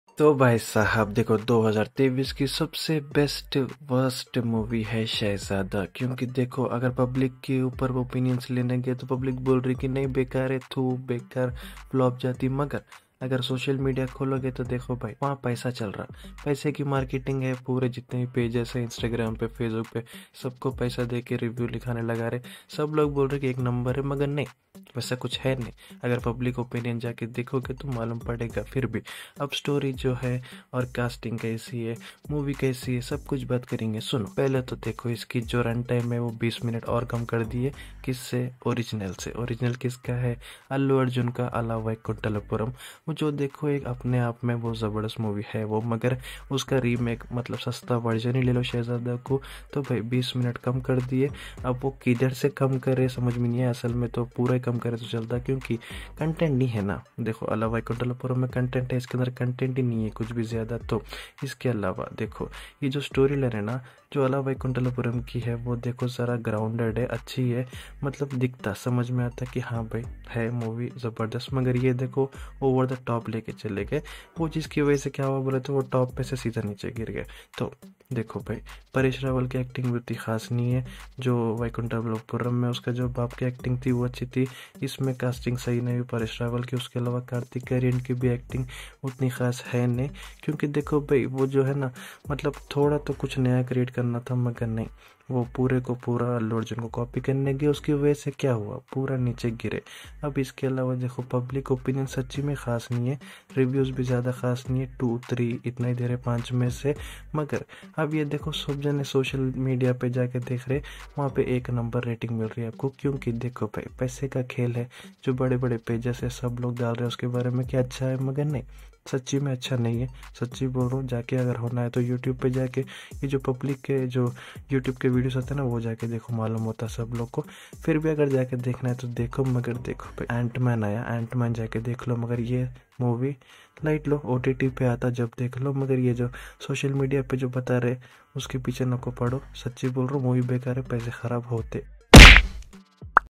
Kartik Aaryan's Shehzada - Overhyped or Underrated?
Kartik Aaryan's latest release, 'Shehzada,' has received mixed reactions from the audience. Despite having a star-studded cast and an interesting plot, the movie failed to impress the audience, and public opinions suggest that it is a flop. However, on social media, many high-profile influencers and pages are posting positive reviews about the movie, claiming it to be a hit.

This has led to accusations of false marketing to sustain and collect box office collections. Many netizens and news channels have expressed their concerns about this marketing strategy. Additionally, people have noted that the original movie of Shehzada, Ala Vaikuntapuramuloo, is the best version, with better acting and storyline. Many people also felt that Shehzada tried to go over the top, but didn't quite make it.

It is crucial to remember that the opinions of high-profile influencers and pages on social media may not always be genuine. False marketing strategies can mislead audiences and harm the movie industry in the long run. It is essential to rely on genuine reviews and form our opinions based on that. As viewers, we have the power to make or break a movie's success, and it is vital to use that power responsibly.